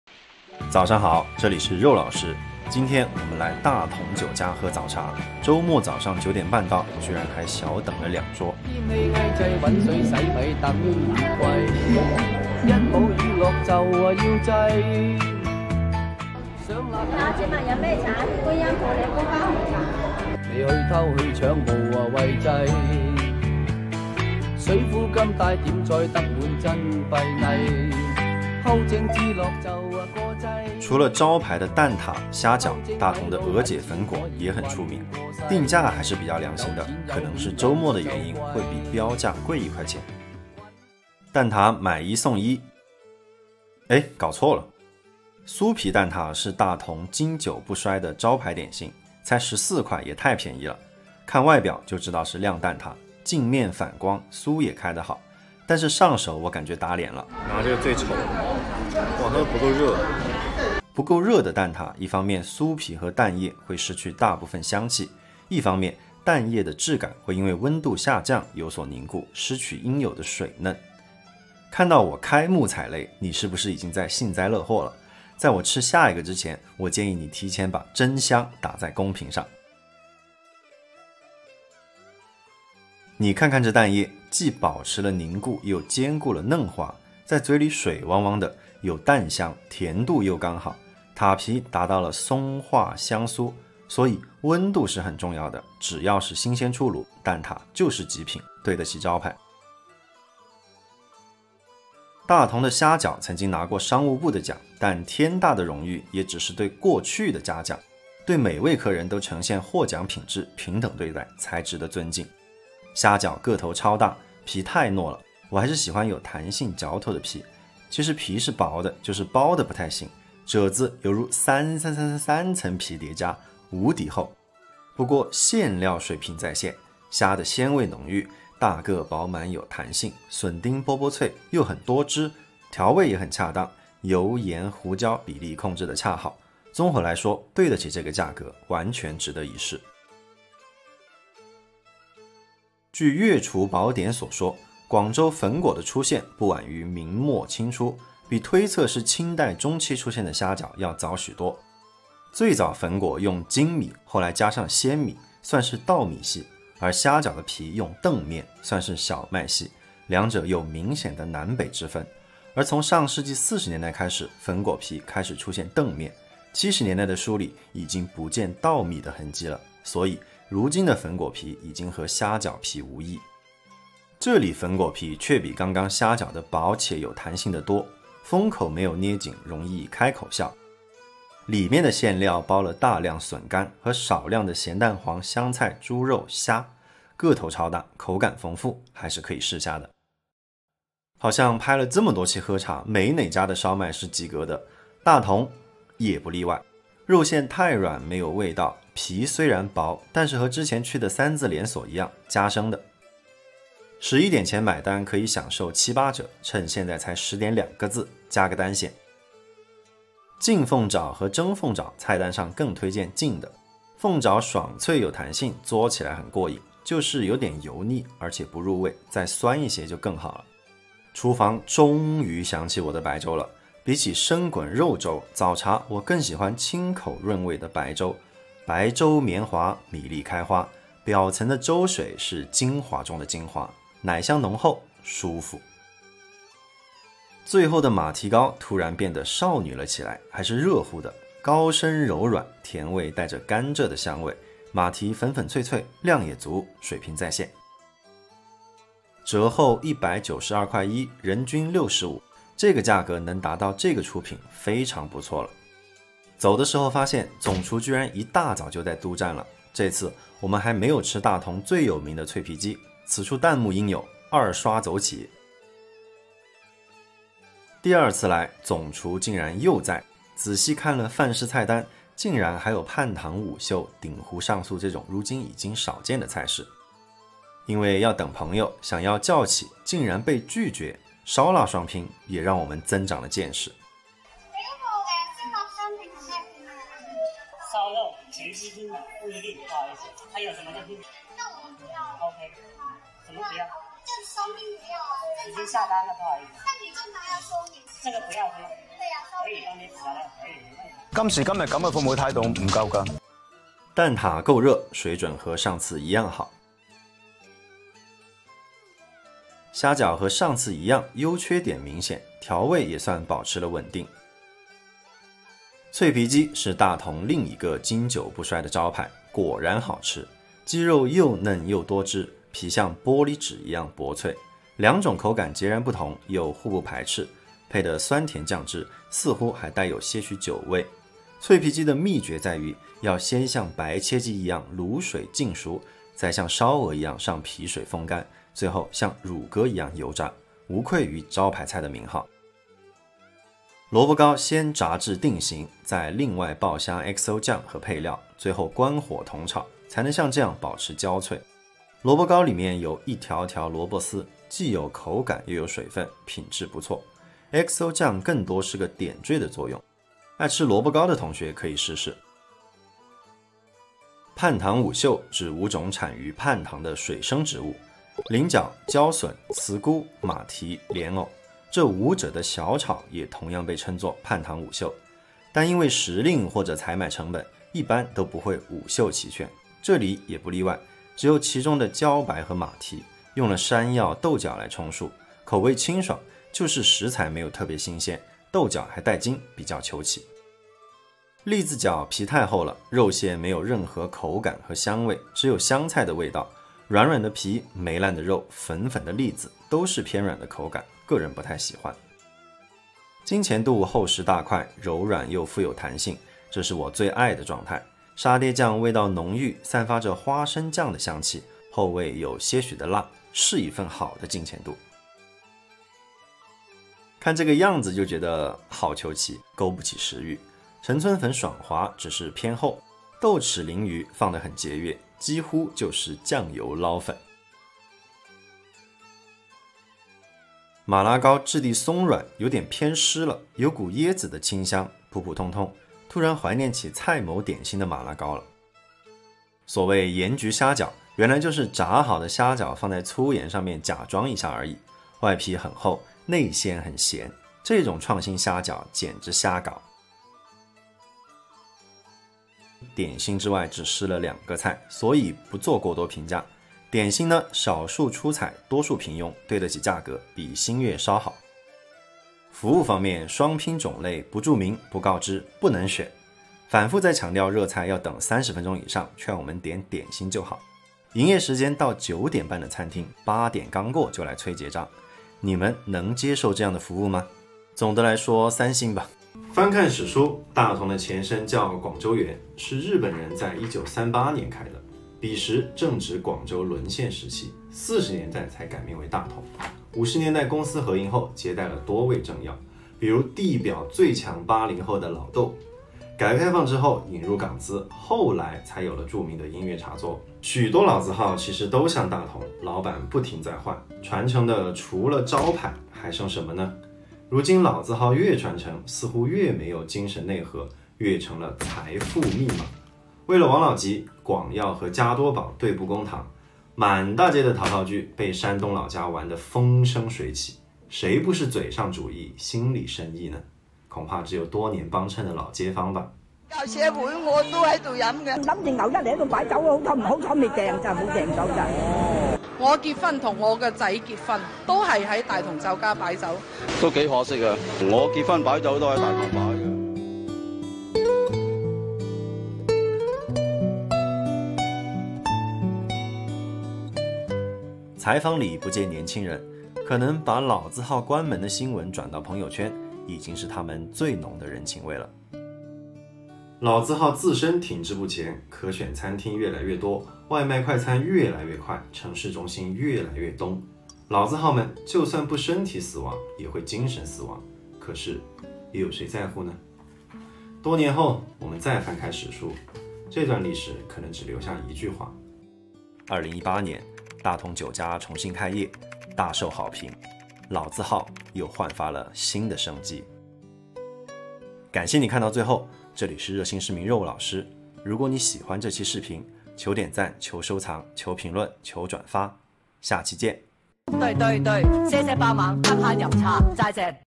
得闲饮茶 | 82年老字号，招牌菜10分，服务0分 | Dim Sum of an 82-year-old Restaurant , Dai Tung
起源于1938的大同酒家，16年倒闭，18年在中信重新开业。大同的蛋挞和脆皮鸡都是一绝，不过服务真的该改改了。
大同酒家/造访2次/天河北路233号中天购物城2楼/人均￥60-￥80/推荐：蛋挞、虾饺、金钱肚、白粥、娥姐粉果
Dai Tung is founded in 1938 but closed in 2016. In 2018, it reopened in CITIC Centre. The Egg Tart and Crispy Chicken are tremendous. However, service here is awful.
（花自己的钱，不事先打招呼，不认识老板，还原真实探店过程，食评仅代表主观喜好。）